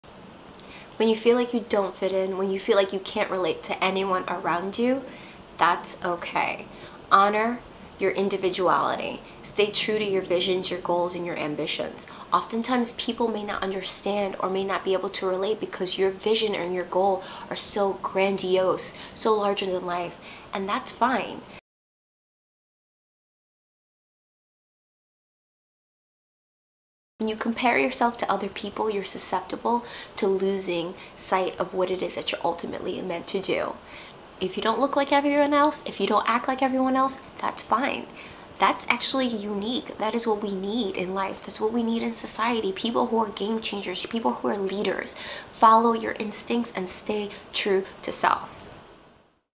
When You Feel Like You Don't Fit In
Happy Monday Beautiful People!

When you feel like you are so disconnected to the rest of society, use that as an opportunity to showcase your special uniqueness. Honor and cherish your individuality. Shine brighter and bolder and don't allow the noise of the world to dilute your essence. Avoid comparing yourself to others. Be who you are meant to be!

Happy healthy living!

Xoxo,

~Mary-Andrée